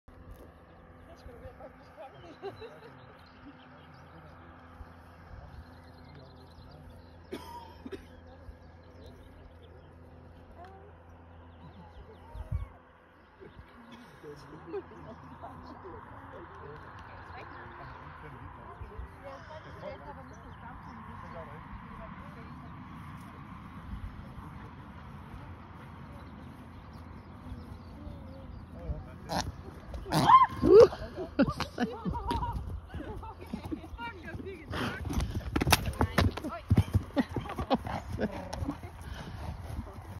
Horse Startled by its Own Fart Sends Rider Flying || ViralHog
Occurred on May 22, 2022 / Gandrup, Denmark

My sister-in-law’s youngster. In this video it’s his first time with the rider (me), I hang on the side for safety, which is ironic because he got scared of the combination of my arm moving and his own fart. No major injuries for me!